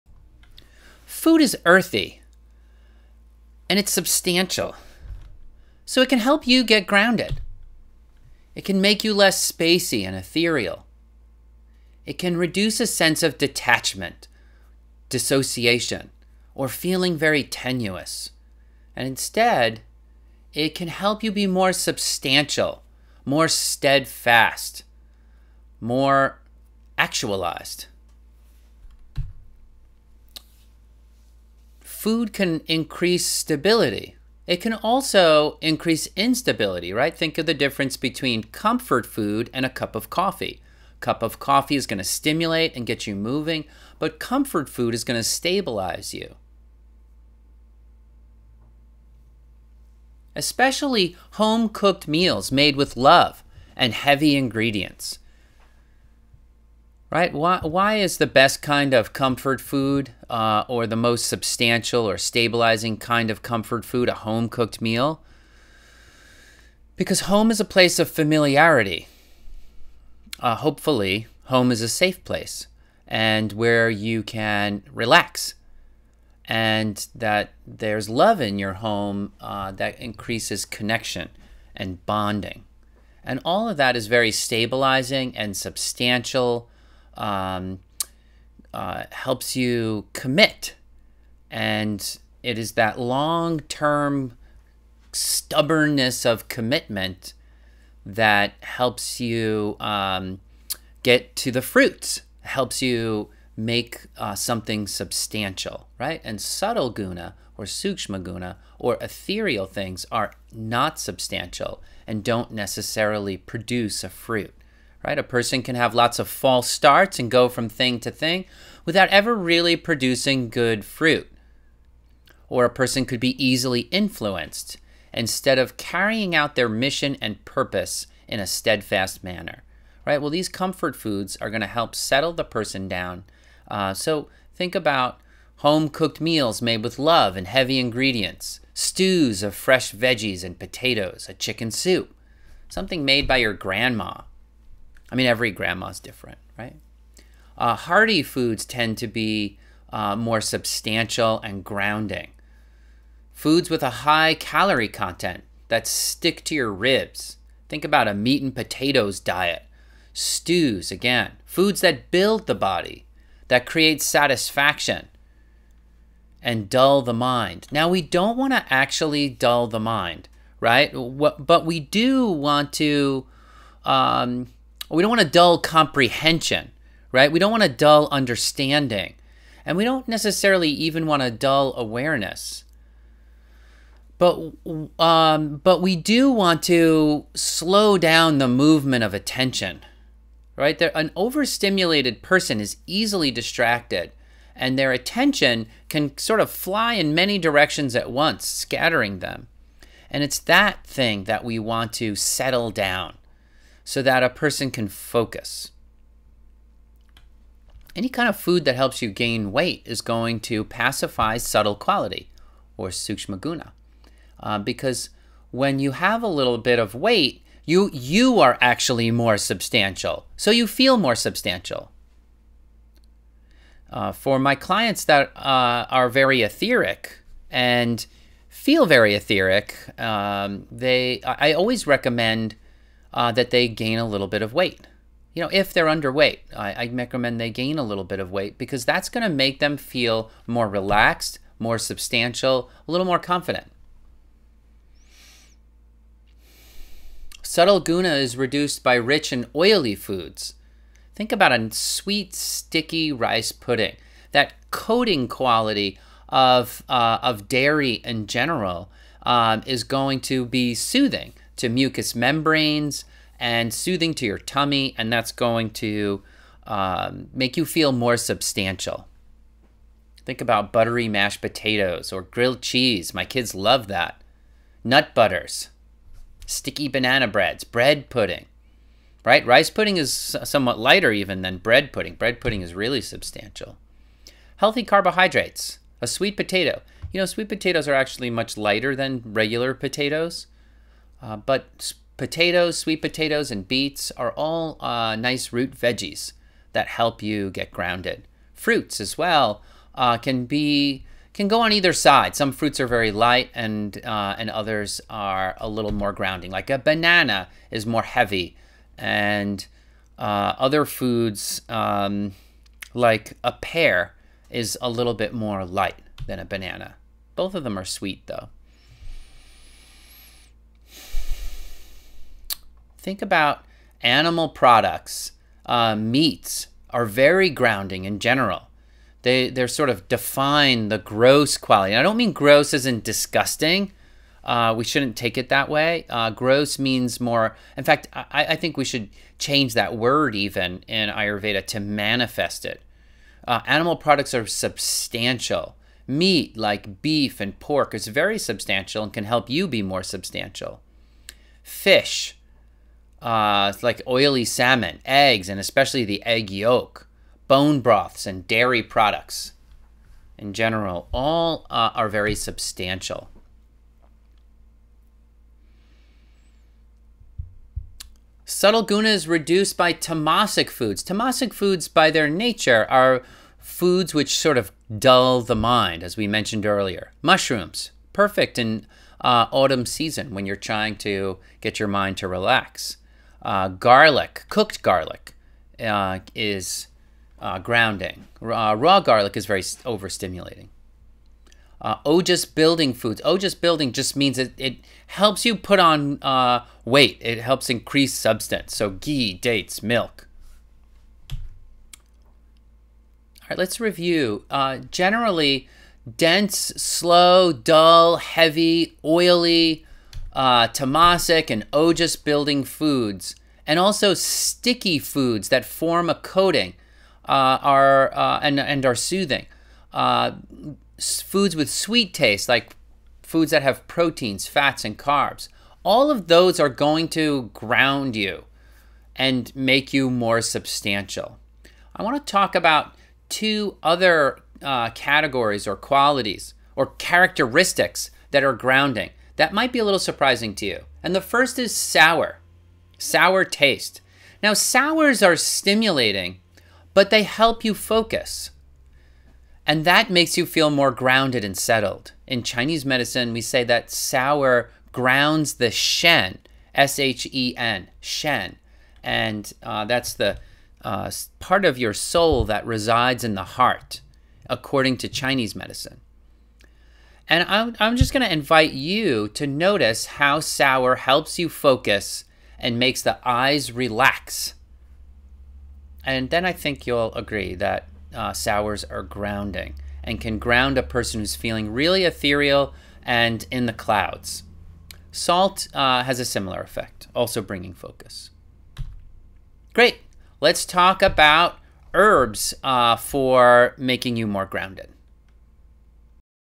Diet to Get Grounded - Getting Grounded With Ayurveda 18 of 20
Ayurveda shows you how to get grounded for people who need it. Ayurveda also provides tools for people who want to be more subtle.